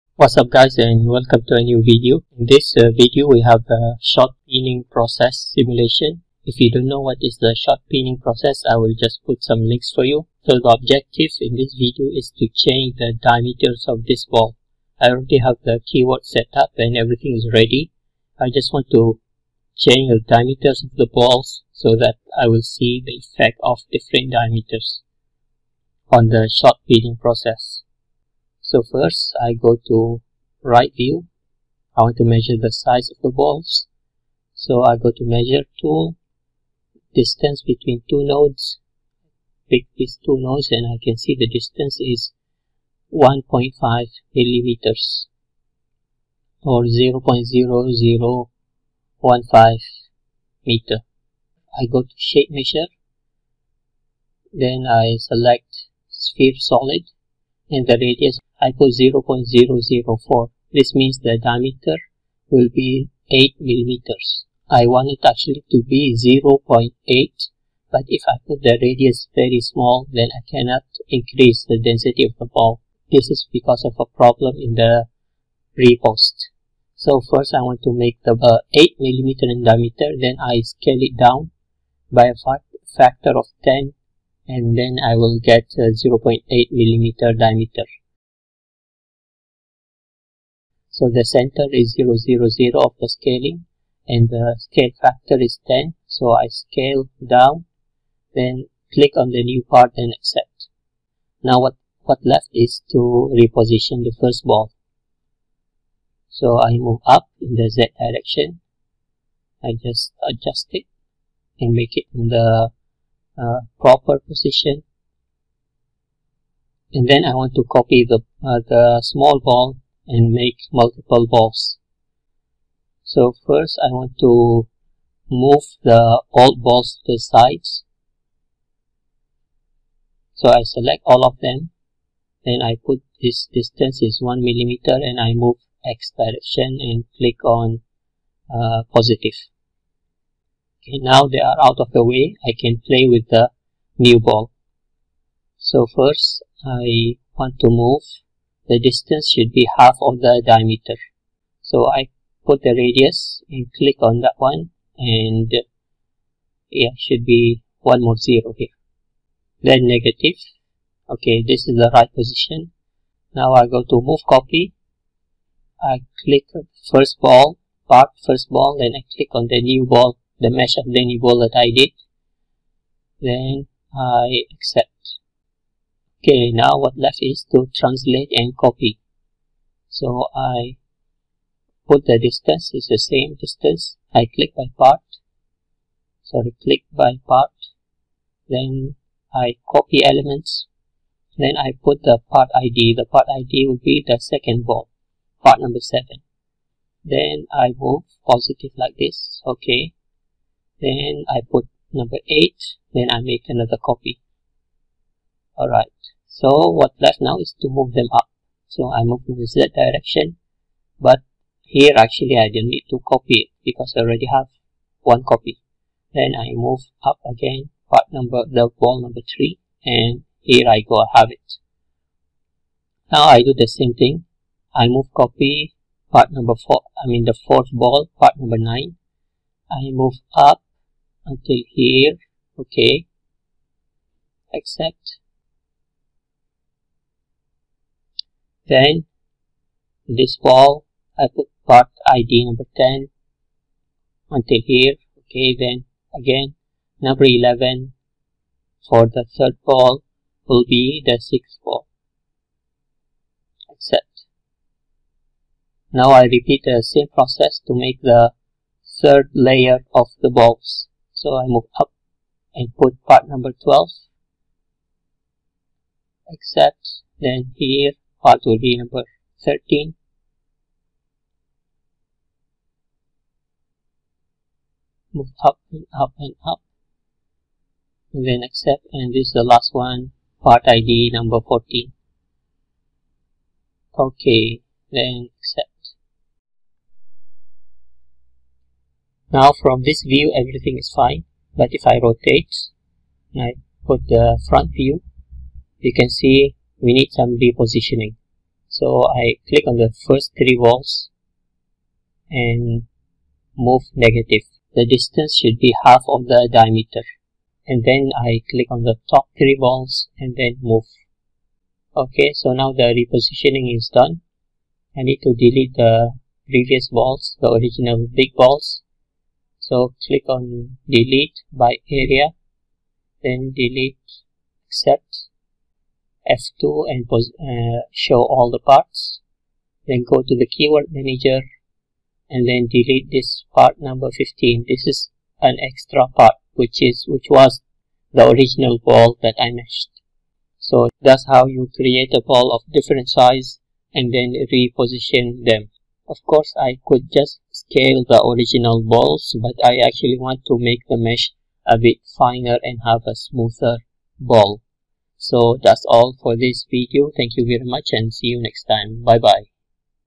Meshing Tutorial: Sphere Mesh Duplicate and Reposition
I made this video helping out a student who is doing a project about shot peening.

I won't discuss the details of setting up the simulation as you can learn that from my previous videos already. Watch here to see how to do simulation of ball on a plate:

This simulation is based on this research article:
Prawoto, Y., Mazein, P. G., Kosterin, A. V., & Ahmad, Z. (2019). Effect of Shot Size in Surface Improvement via Shot Peening; Analytical, Modeling and Experimental Approaches. Int J Metall Met Phys, 4, 033.